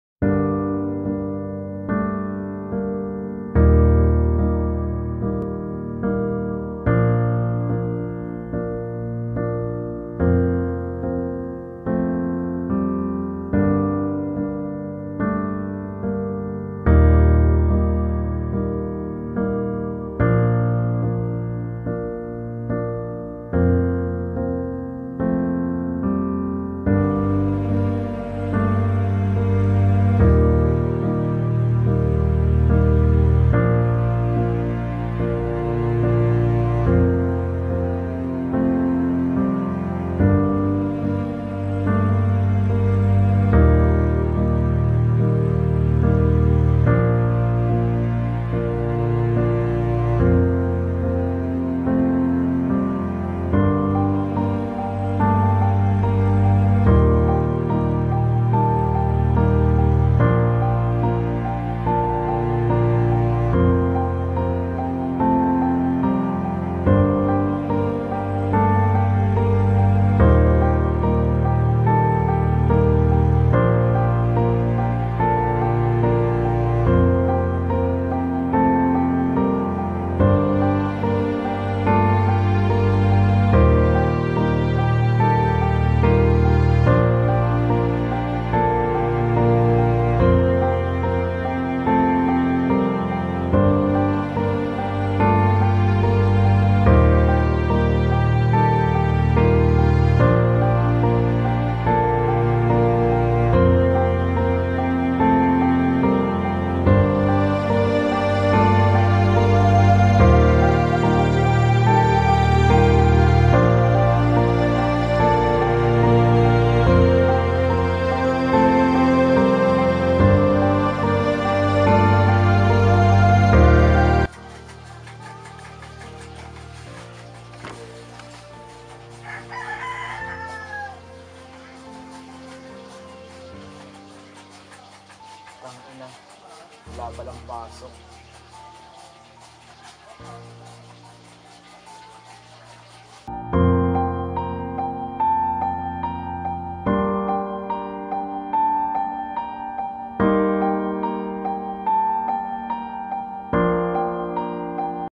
WALANG PASOK
dahil sabi ni mayor na walang pasok mga high school